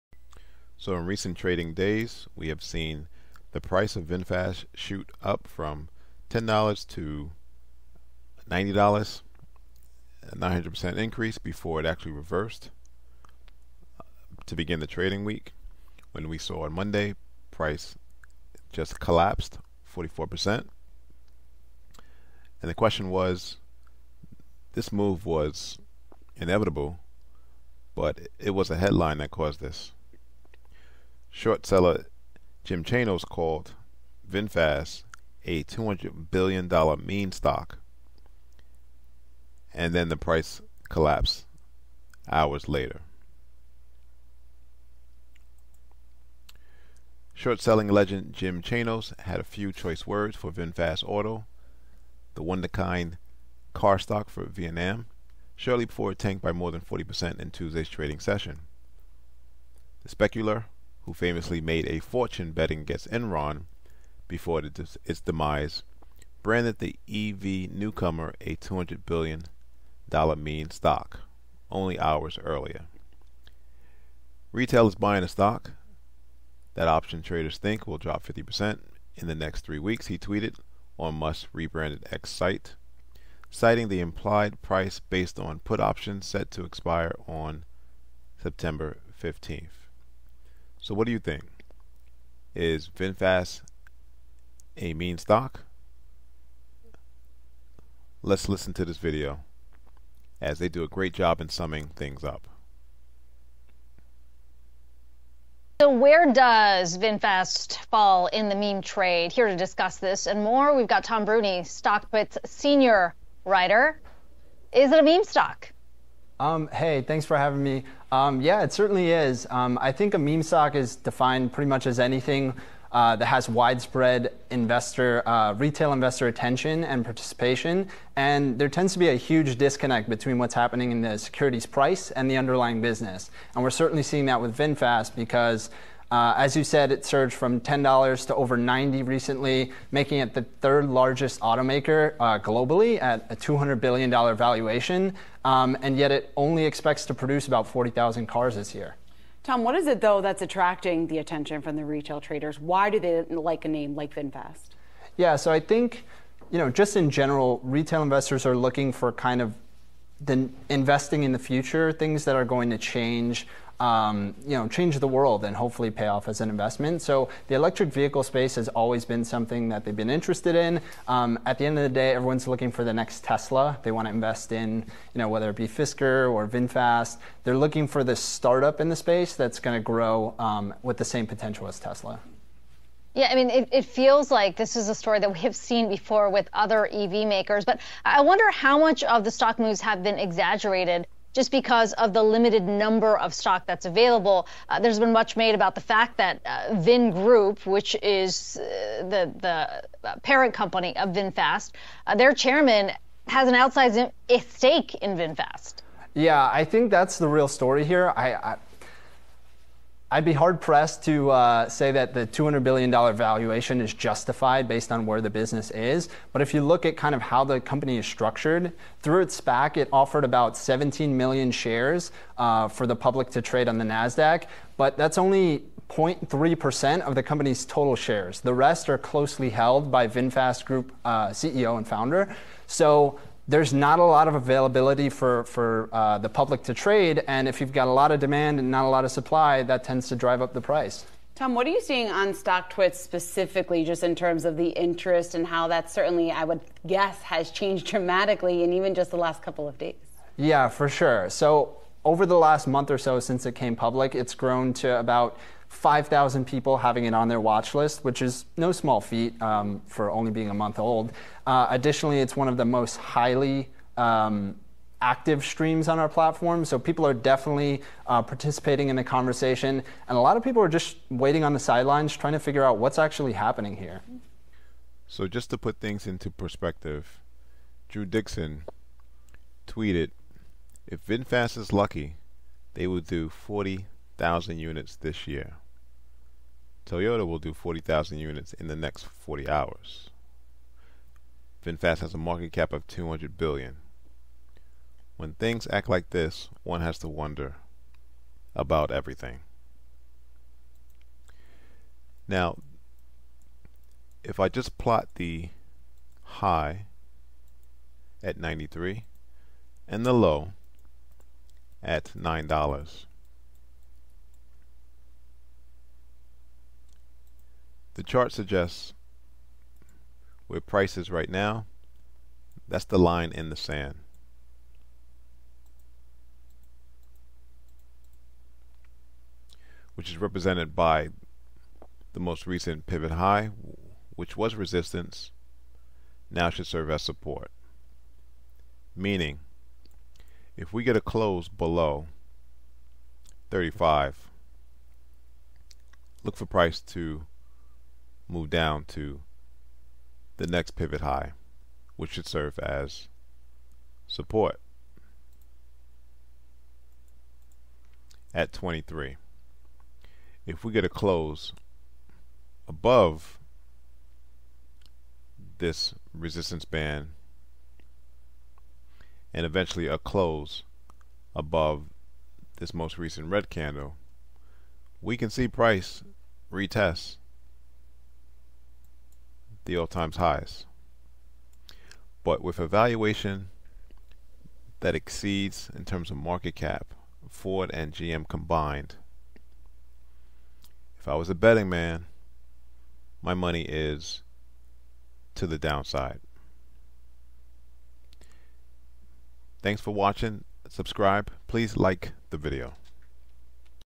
Unmasking VinFast Stock: Is It a Meme Stock or Hidden Gem?
Do you think VinFast is a meme stock? Check out this video to find out! In this video, we'll take a look at VinFast and see if it's worth investing in.

Legendary short-selling Before VinFast Auto, the Vietnamese car stock, fell more than 40% on Tuesday, Jim Chanos had some choice words for it.

The speculator who made a fortune betting against Enron called the electric vehicle newcomer a “$200 billion meme stock” hours before.

According to a tweet on Musk's renamed X site, retail is buying a stock that traders predict will plummet 50% in three weeks. The suggested price is based on put options expiring on Sept. 15.

Whether Chanos was hinting he was shorting VinFast like he did Tesla is unknown, but investors follow the Wall Street veteran for trading tips.

Fortune contacted Chanos and VinFast for comment, but neither responded.

The startup's public debut as a Vietnamese automobile was a fairy tale.It listed this month through a special purpose acquisition company instead of an IPO.

This despite VinFast's six-year age, 18,700 electric vehicle sales, and origin in a country that built half as many automobiles as Morocco last year.

Worse, it was known for creating the worst new U.S. EV this year.  Customers were so furious that it rushed developed a program to pay owners in June.

VinFast's overvaluation at the start of the week is shown by its $80 billion market value loss on Tuesday and its standing as the world's third most valuable carmaker after Tesla and Toyota.

Morningstar data reported by Yahoo Finance shows Toyota's market cap is below its trailing 12-month revenue. This is fair for an established industry leader. In contrast, investors discount GM and Ford more.

Rivian, Nio, and Xpeng are growth stocks worth up to six times their revenue. Under Elon Musk, Tesla charges a bigger premium as investors expect more high-margin earnings from its self-driving software, FSD.

In contrast, VinFast traded at 350 times its yearly sales. This calls into doubt its market value. Short-sellers may be tempted to bet big on the air being devalued, but it's not that easy.

Chanos or anyone else prepared to bet bearishly may have trouble finding a prime broker to loan out the shares.

Pham Nhat Vuong, chairman and founder, owns 99% of the stock and was Asia's second richest tycoon this week because to VinFast.

Shorting VinFast may demand too much collateral because the bet could go south at the slightest purchasing push.

Chanos's indirect advice to hunt for options market opportunities may be easier for investors ready to risk money on such a volatile company.

Whether you think VinFast is a good investment or not, I hope you'll check out this video and let us know what you think. We'd love to hear your thoughts!